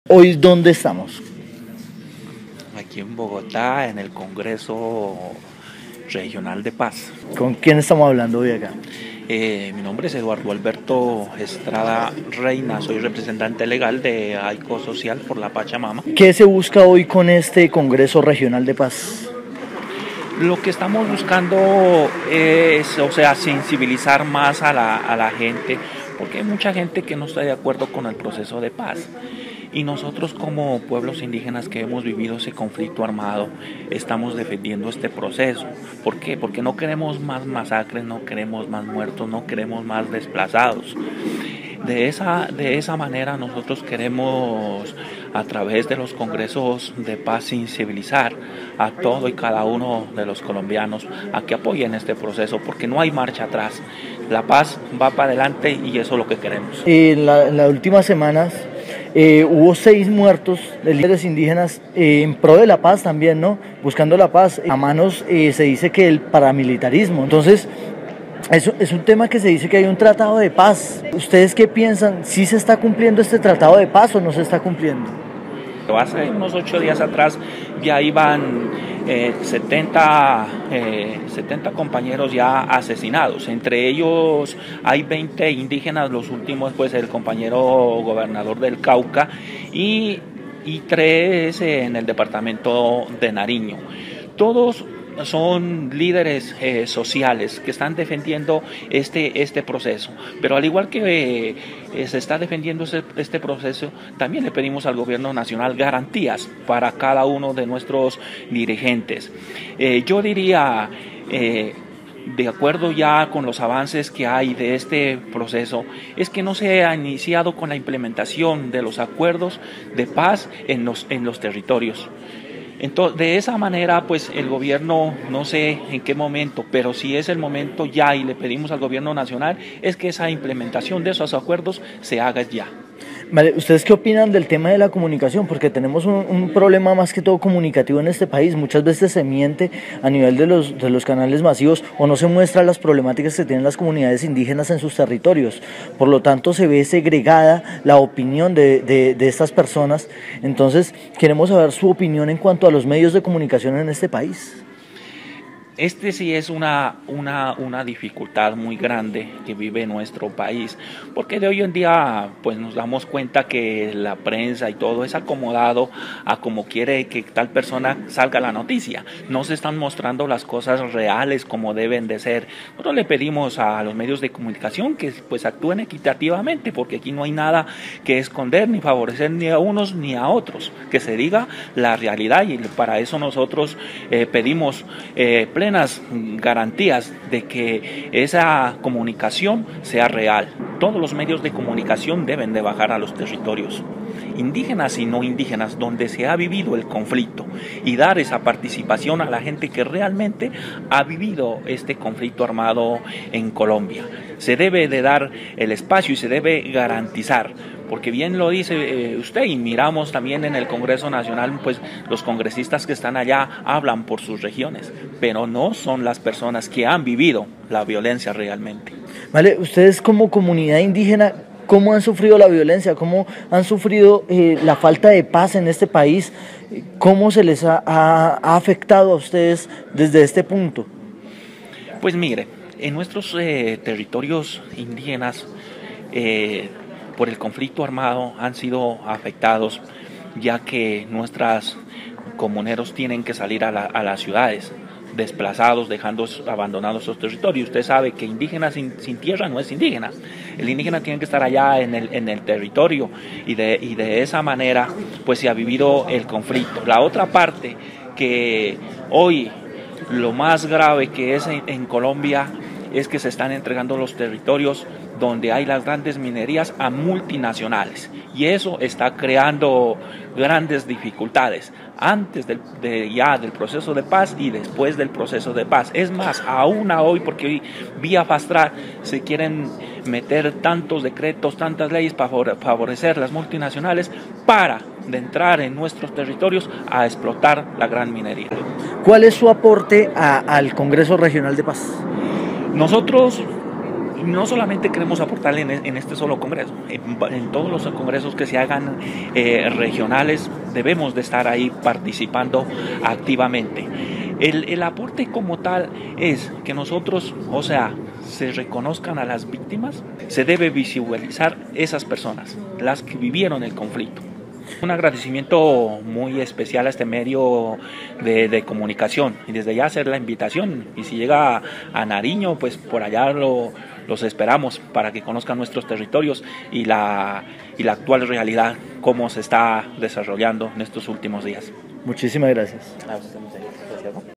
CONGRESO REGIONAL DE PAZ BOGOTA - CUNDINAMARCA
ASÍ SE VIVIÓ EL CONGRESO REGIONAL DE PAZ BOGOTA - CUNDINAMARCA, QUE SE DESARROLLO EN LA LOCALIDAD DE ENGATIVA
ENTREVISTA AL LÍDER SOCIAL INDÍGENA  EDUARDO ESTRADA